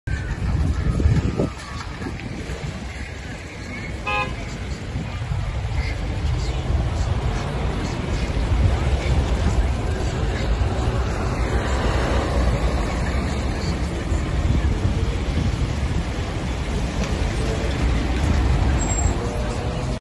Центральне перехрестя у Рівному
Не працював світлофор. Відео - Радіо Трек.